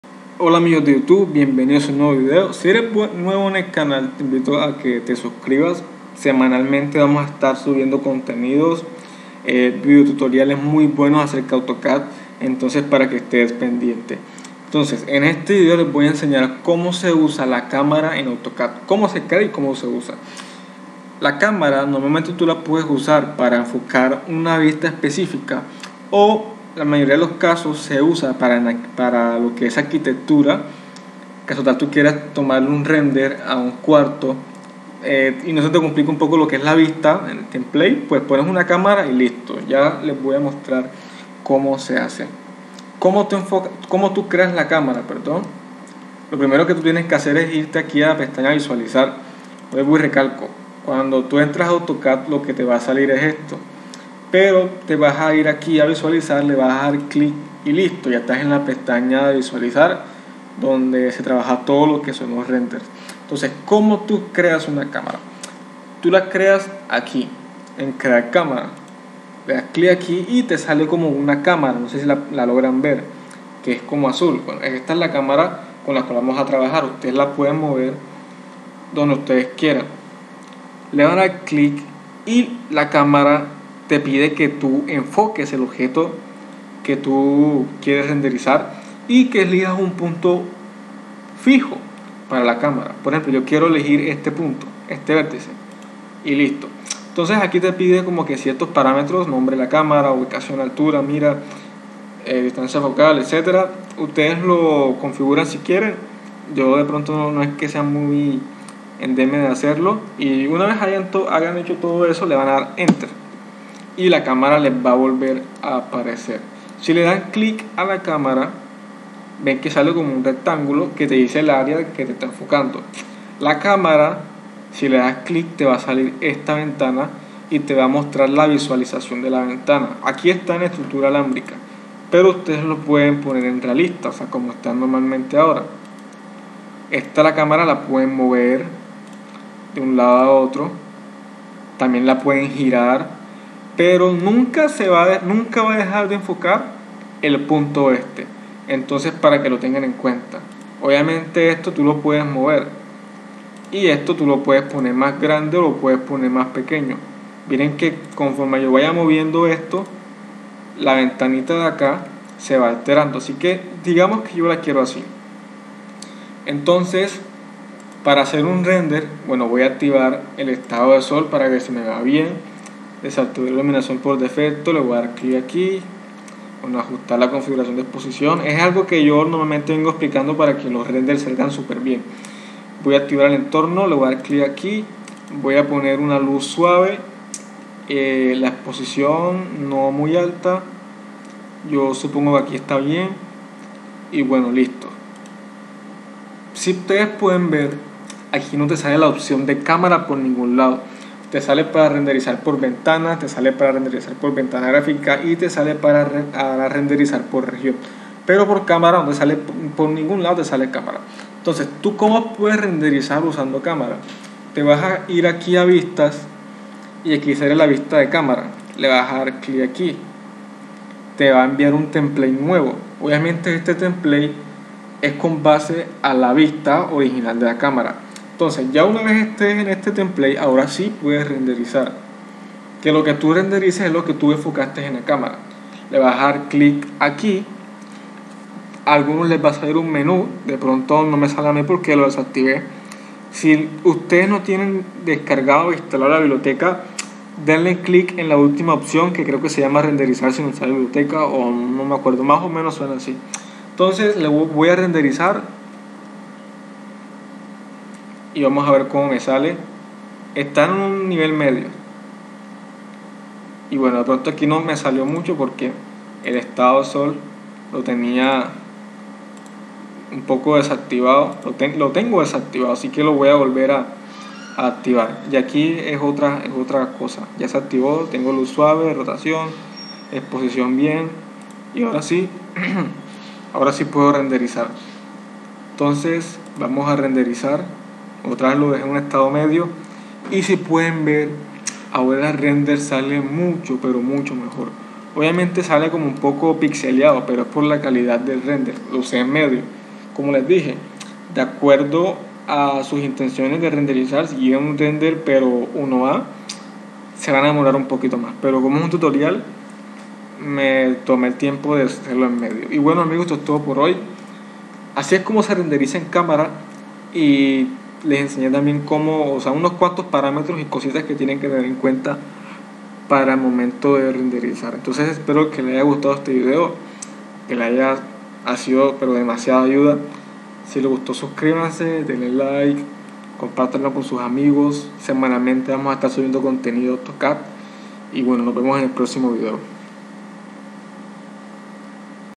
AutoCAD 3D -14- Renderizar con una cámara en AutoCAD
Bienvenidos a un nuevo video, Renderizar a través de una cámara es una de las formas para renderizar de una forma más enfocada y más precisa, se las recomiendo y a qué les explico cómo se hace y también les muestro ciertas cosas que hay que tener en cuenta. espero que les guste el video, no olviden dejar su like y suscribirse :D